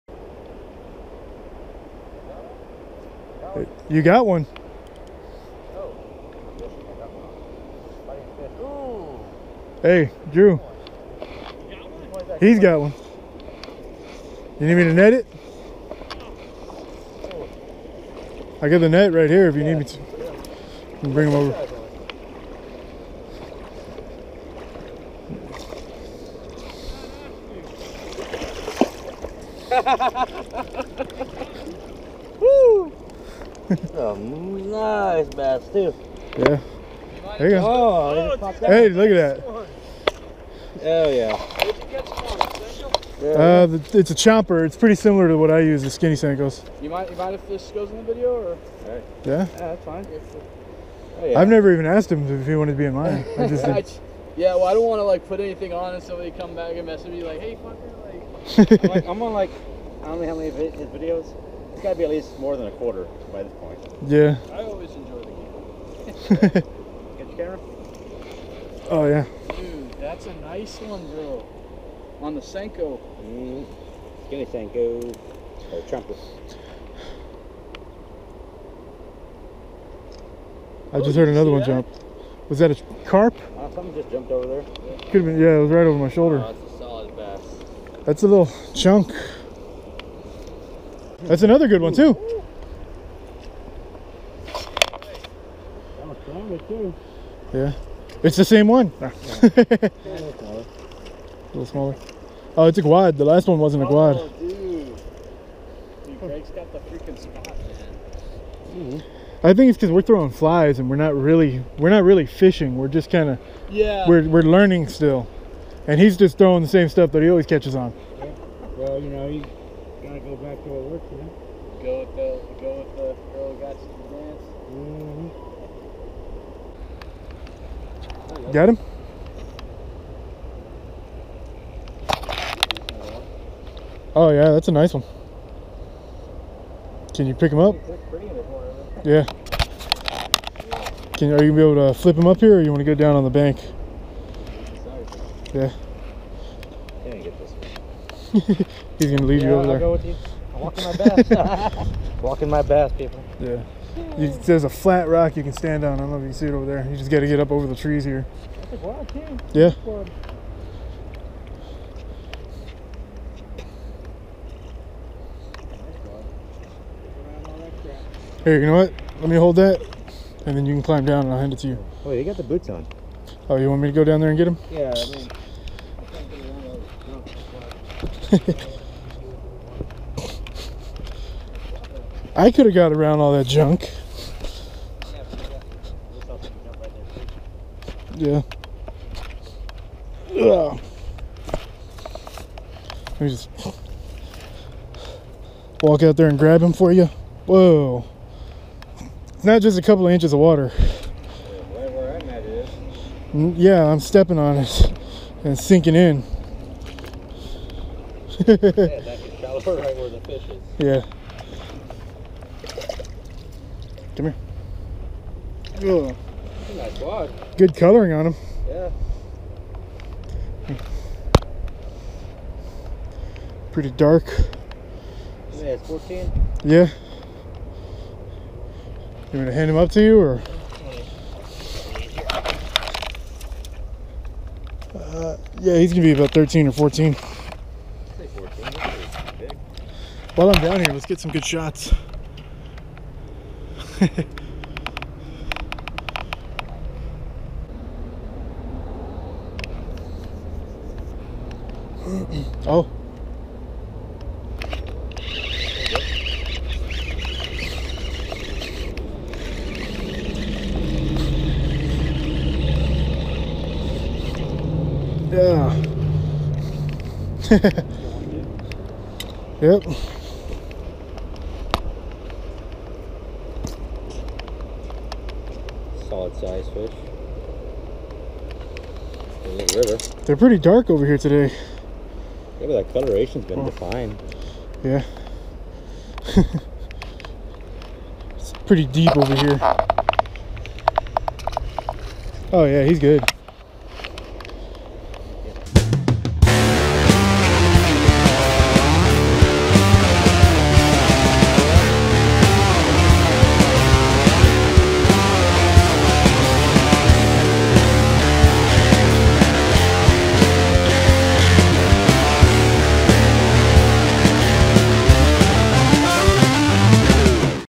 San Antonio River Bass Fishing - Winter 2018
Filmed December of 2018. Pulled these clips out of the archives since I haven't had the chance to get out lately.

Hey Mr. Moneybags, why don't you drop a dollar in my tip jar?: